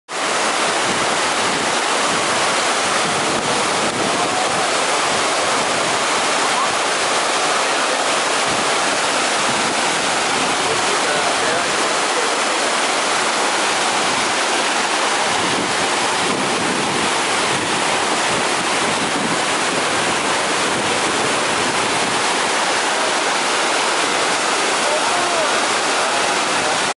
Khlong Lan Waterfall @ Kamphaengphet, Thailand
the astonishing 9 storey waterfall in Kamphaengphet in Thailand..